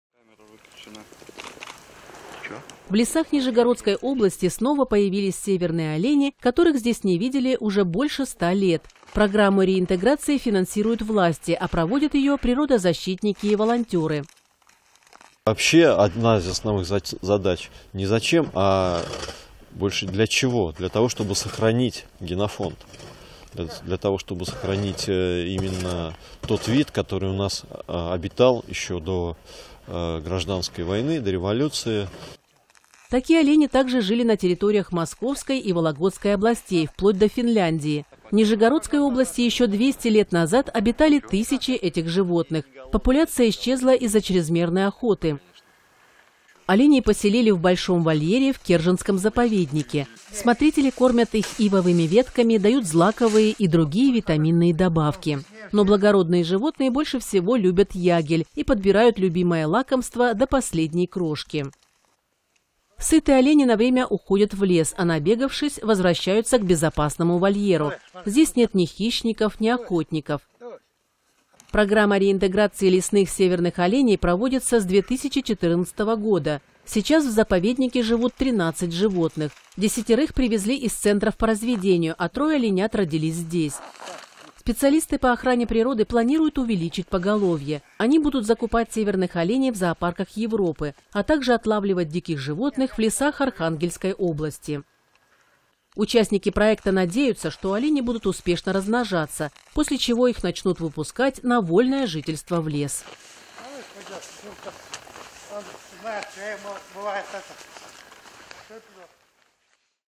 В нижегородские леса возвращают северных оленей
В Нижегородской области возвращают в дикую природу популяцию северных оленей. Эти животные исчезли там 100 лет назад. Программу реинтеграции помогают проводить волонтёры.